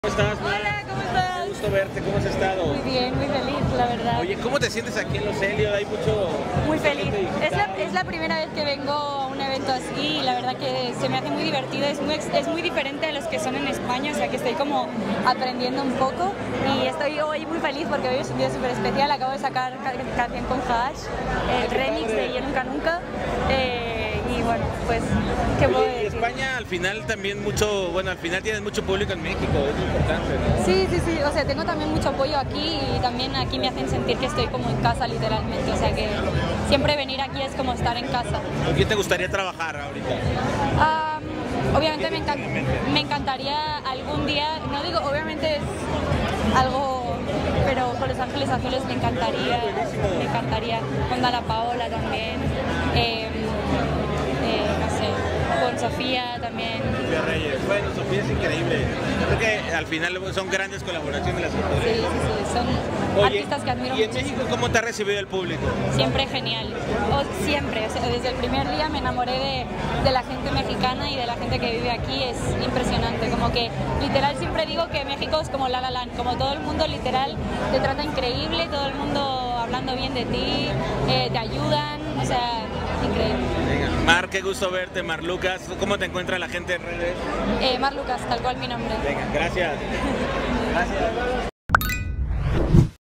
MAR LUCAS esta feliz de abrirse paso en la música en México y sueña con cantar con DANNA PAOLA
La cantante española Mar Lucas estuvo en México y fue invitada especial a los Eliot Awards ahí hablo de su ultima canción con Ha-Ash y dijo que tiene el sueño de cantar con Danna Paola.

SÍGUEME